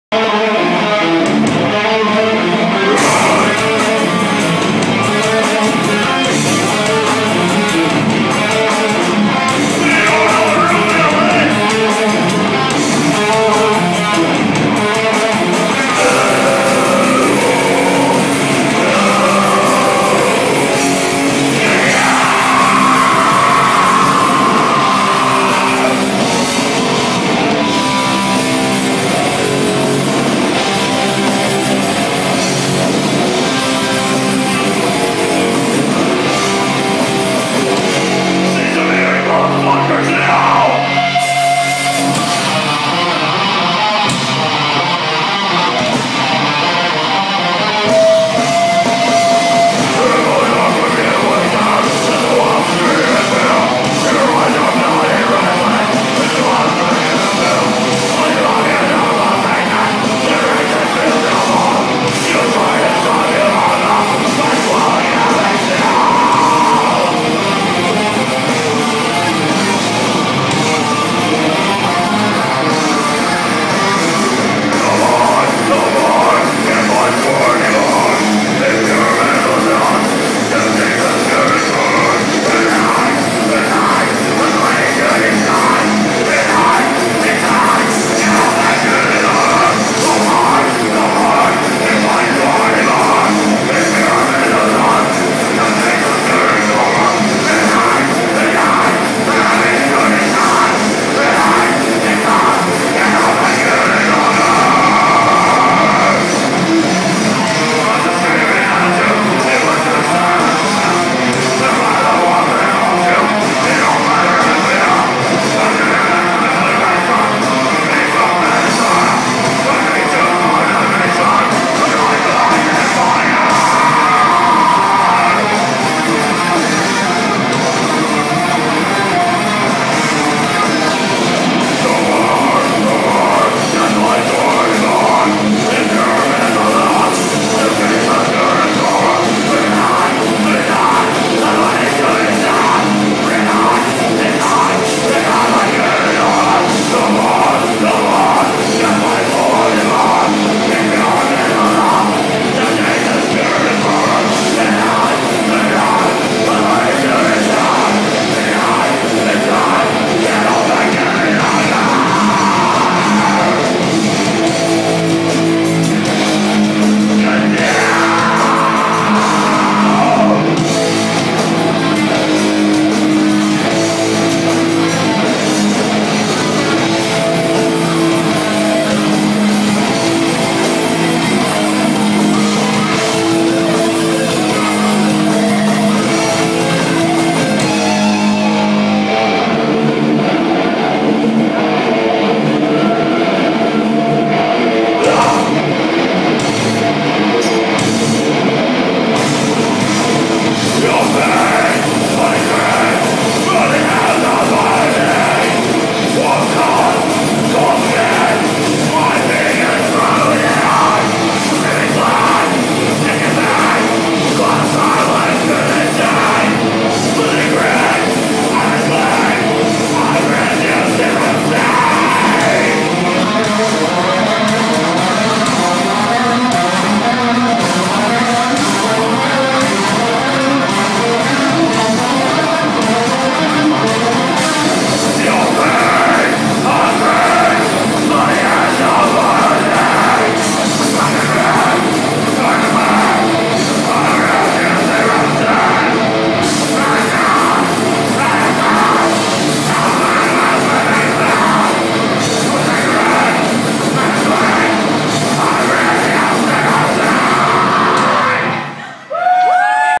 The Machine Shop
These guys were awesome! Got to chat with the frontman & what i think was his girlfriend after the show & they were awesome.